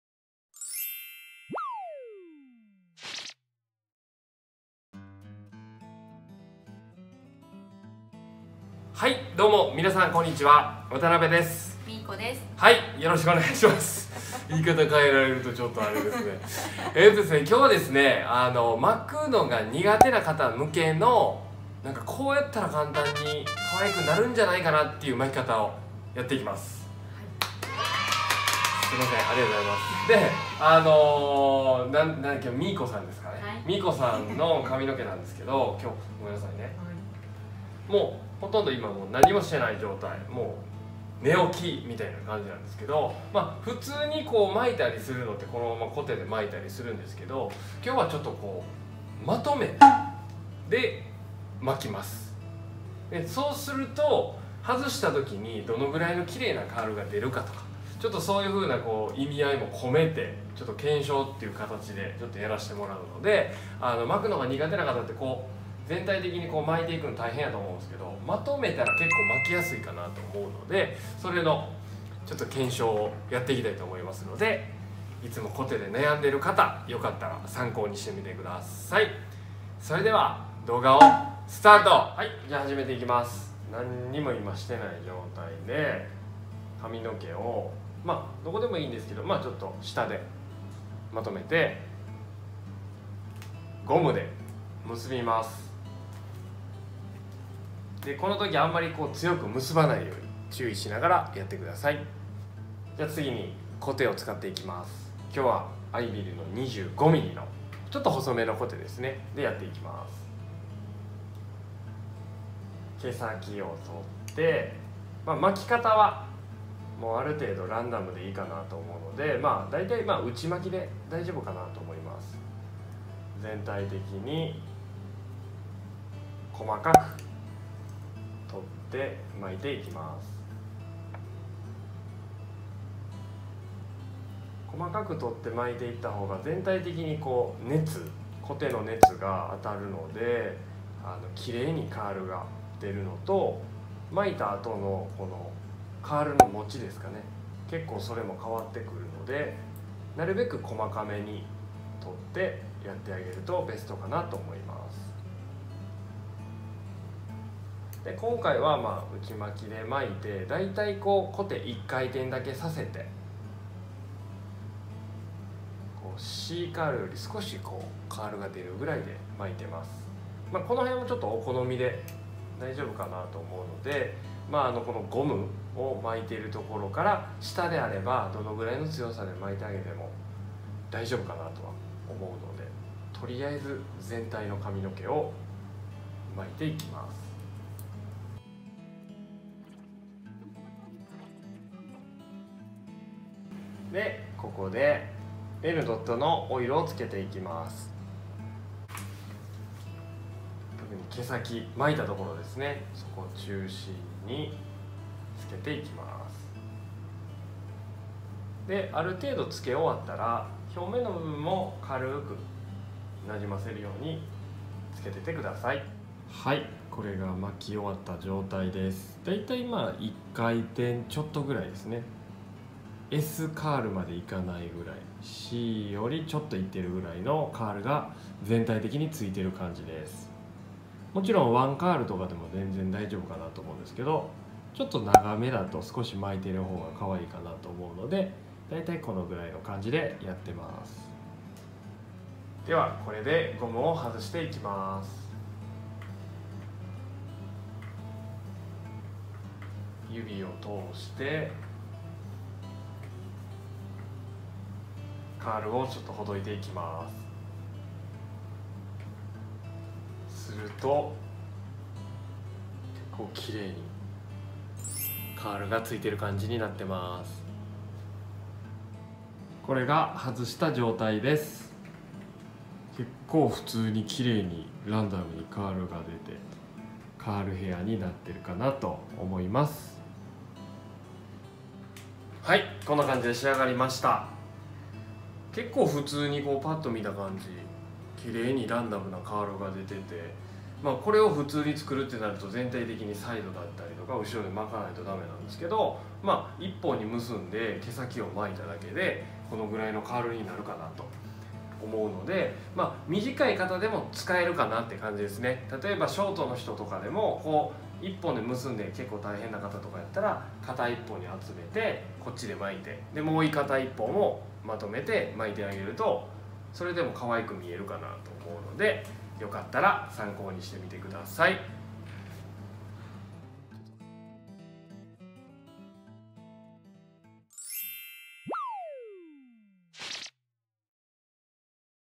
巻くのが苦手な方へ、まとめてから巻くと誰でも可愛く出来ます♪SALONTube 渡邊義明 Hair styling Hair arrangement 头发 헤어
巻くのが苦手な方へ、まとめてから巻くと誰でも可愛く出来ます♪

いつもお悩みの方、必見ですよ〜♪

【使ったもの】
PUバンド

美容動画

Hair styling
Hair arrangement
头发
헤어